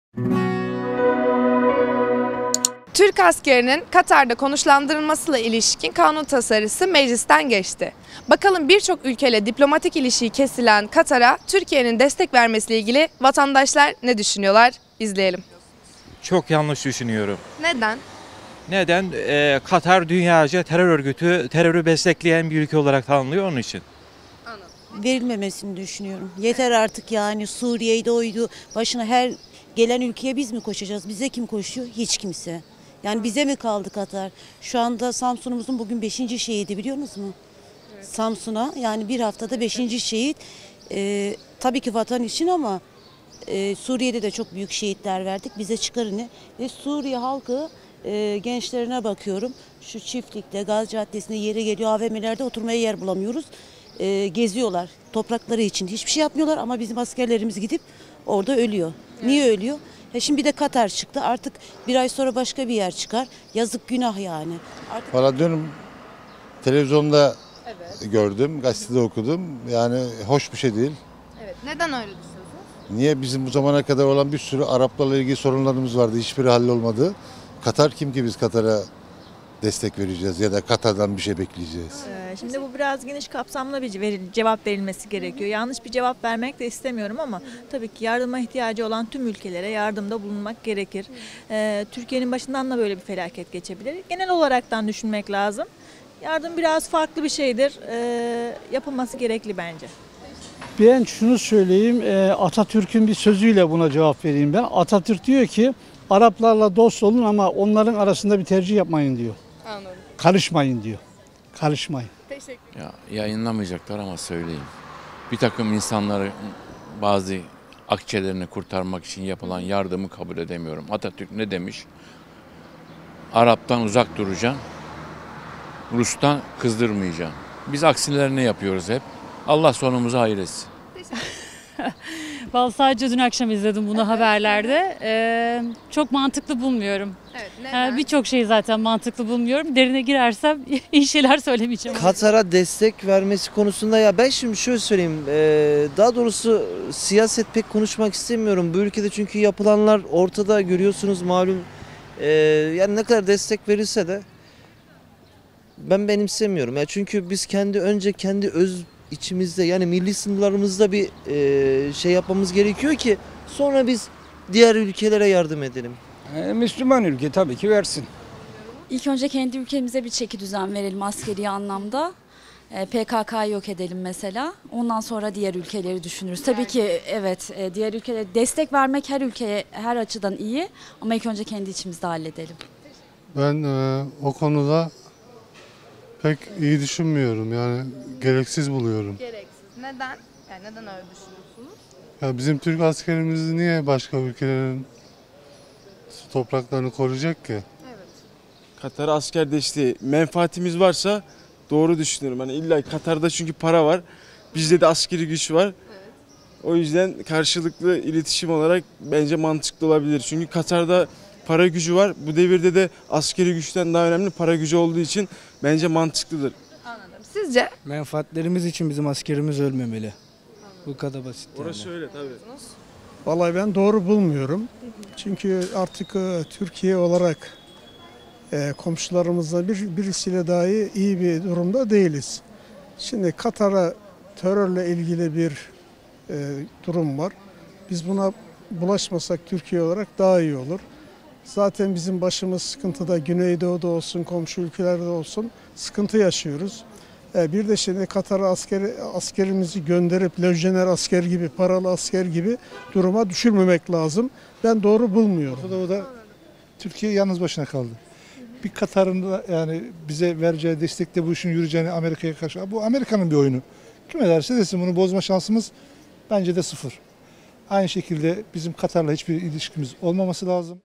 Katar'a destek verilmeli mi?
Samsunhaber.com olarak bir çok ülke ile diplomatik ilişkisi kesilen Katar'a Türkiye'nin destek vermesi hakkında ne düşündüklerini sorduk. İşte gelen cevaplar...